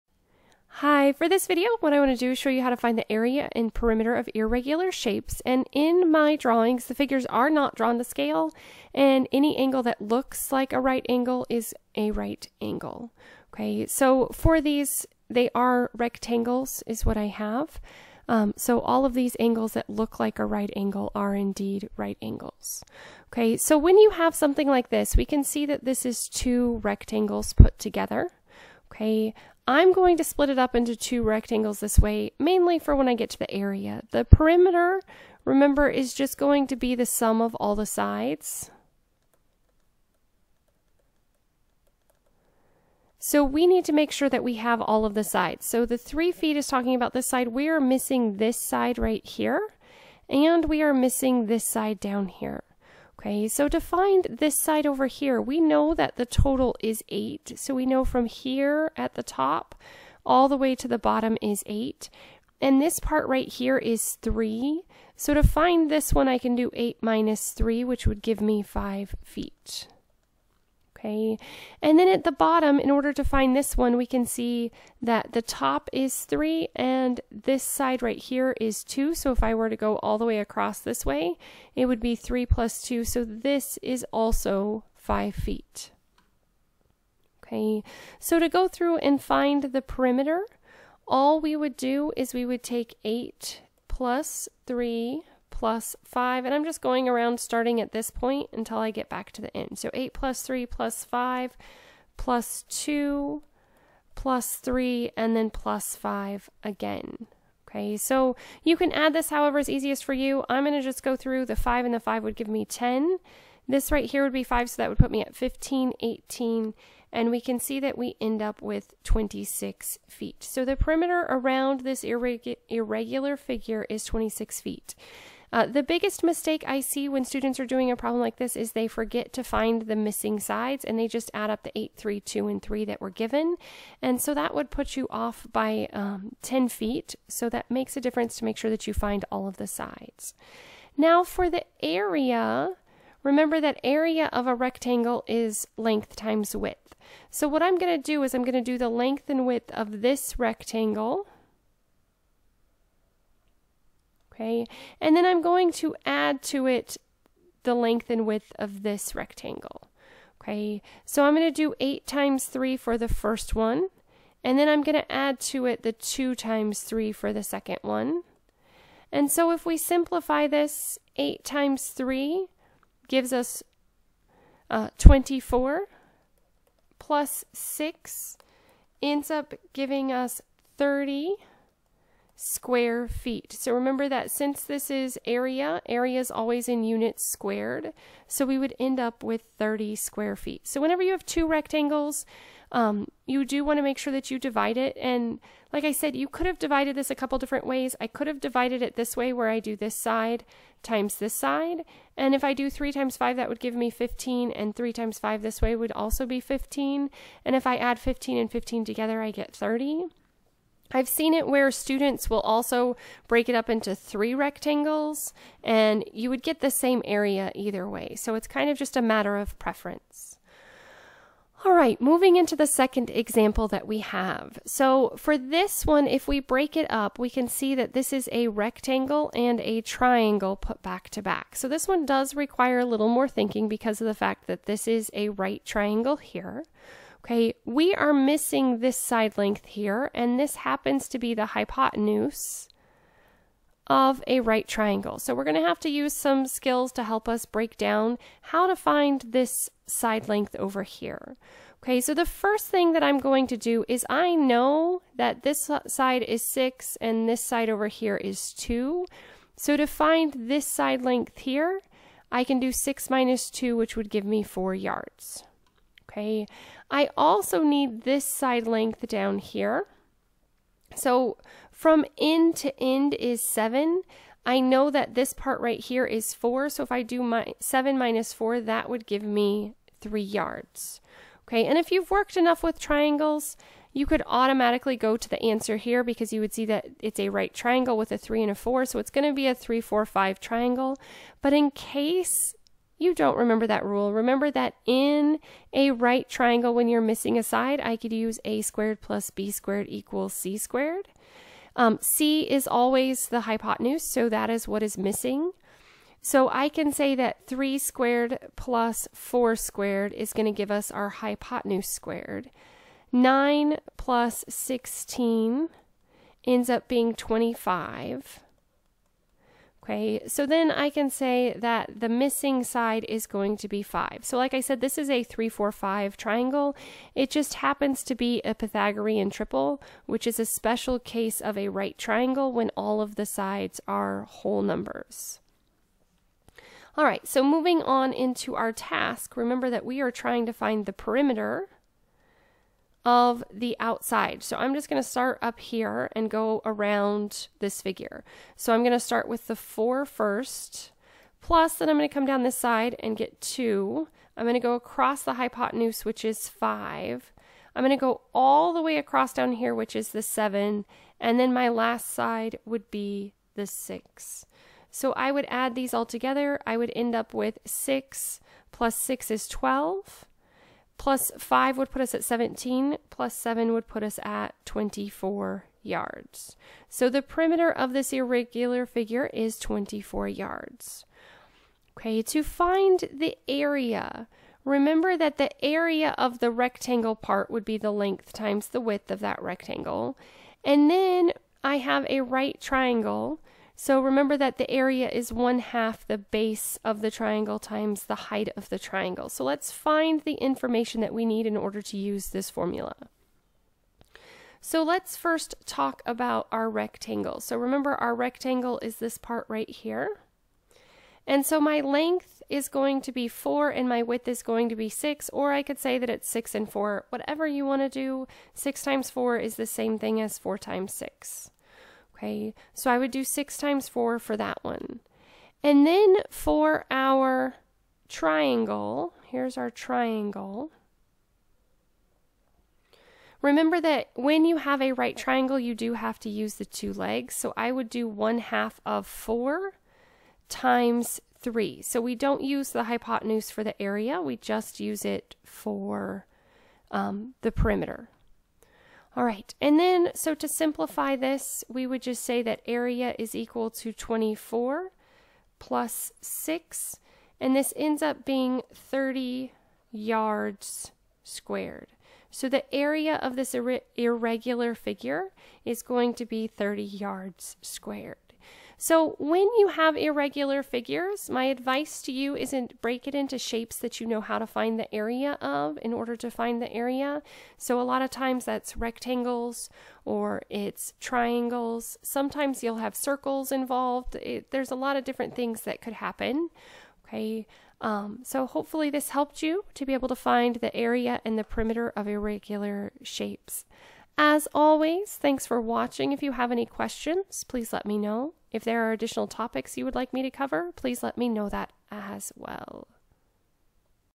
Find Area and Perimeter of Irregular Shapes
This video shows how to find the area of an irregular shape.  In order to do this, it is best to split the shape into shapes that you can easily find the area of. For example, you might be able to find the area of two rectangles or a rectangle and a triangle.